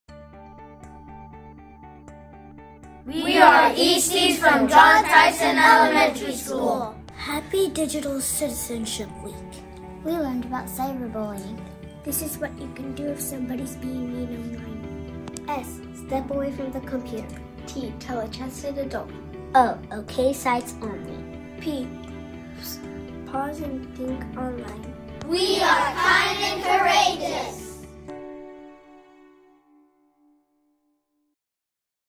sprEAdpoSiTivity Online
This video is about what 3rd grade EAST students at John Tyson Elementary School have learned about being kind online during Digital Citizenship Week.